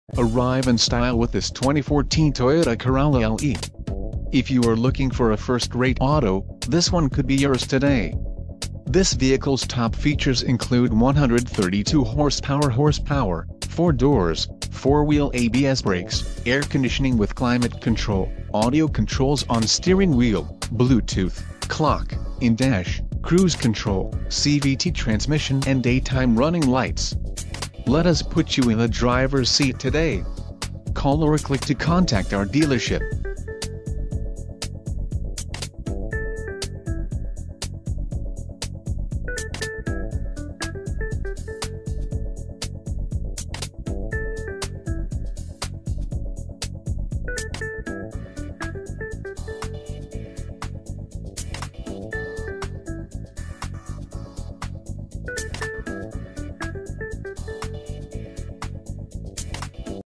2014 Toyota Corolla LE - Serra Toyota Decatur - Decatur, ...
2014 Toyota Corolla LE Condition: Used Mileage: 39402 Transmission: CVT (Continuously Variable) Exterior Color: Gray Stock